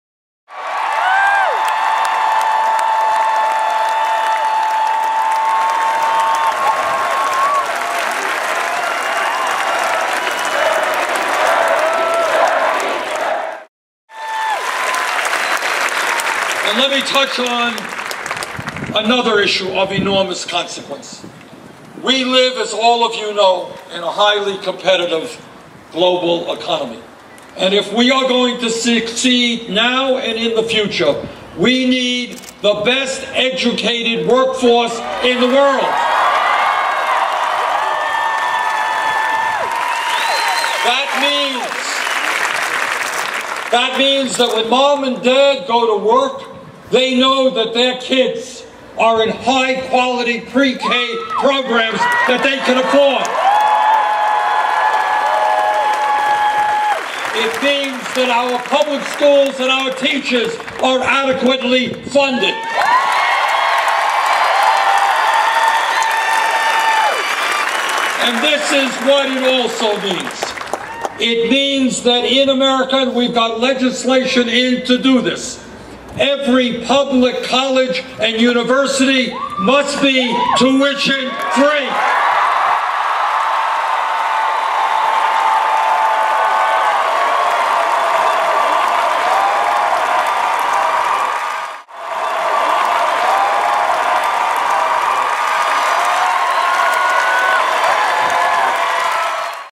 Bernie Sanders in Tucson: Education & Free College Tuition
This is an excerpt from Presidential Candidate and Vermont Senator Bernie Sanders hour-long speech before 11,000 Tucsonans. In this clip, Sanders talks about in importance of education. His call for free tuition for public universities got the loudest applause of the night.